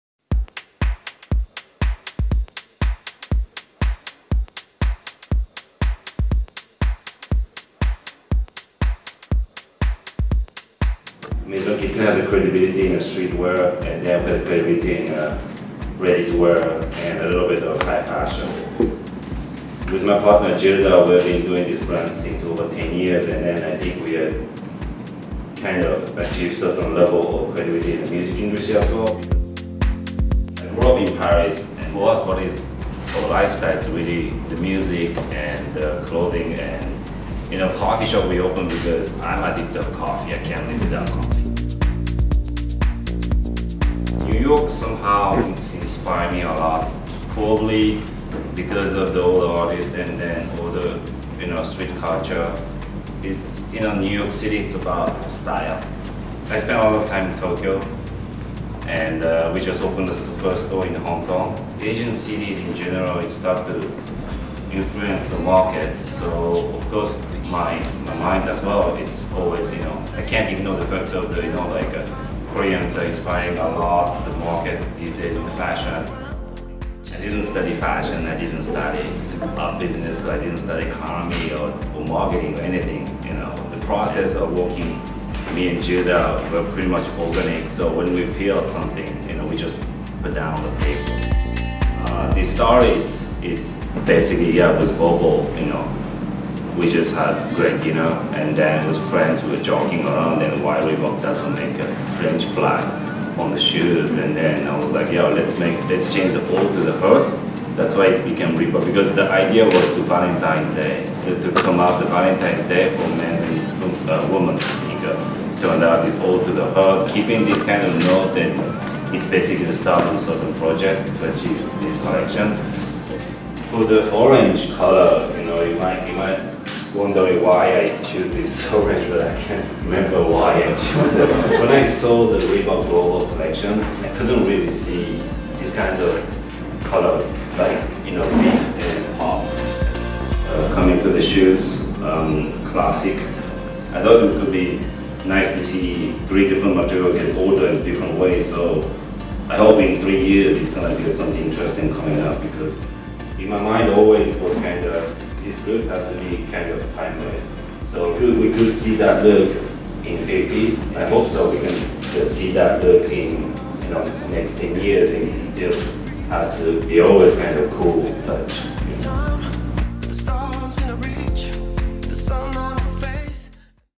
Masaya Kuroki on Cafe Kitsuné, Baseball and Reebok Classics
Never far from exciting ventures that span the realm of fashion, art and music, Kitsune continues its global invasion -- following recent outposts in Hong Kong and New York. The brand's latest venture sees a follow-up from its Parisian-inspired Reebok Classics collaboration, unveiled in February. Teaming up again with the longstanding sporting imprint for 2015 fall/winter, the two brands worked up a capsule collection inspired by baseball. We recently traveled to Tokyo to catch up with Kitsune co-founder Masaya Kuroki, who shares with us his love for coffee, and further explains how this affinity contributes to the brand's overarching aesthetics in the often ambiguous realm of "lifestyle." He also lets us in on how the Reebok Classic collaboration came to fruition and sheds light on his Japanese heritage.